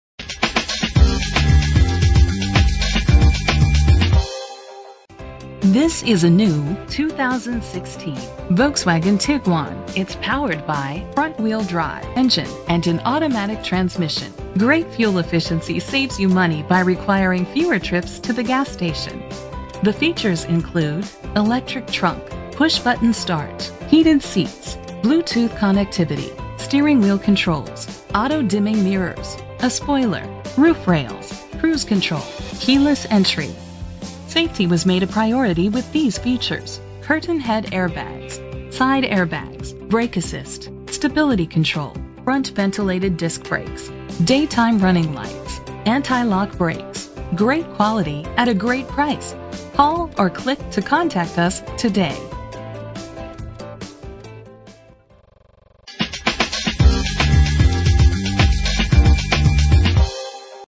2016 Volkswagen Tiguan - Mobile AL
Year: 2016
Make: Volkswagen
Model: Tiguan
Trim: S
Engine: 2.0 L, 4 cylinder
Transmission:
Color: Gray
Mileage: -1
Address:
1525 Beltline Hwy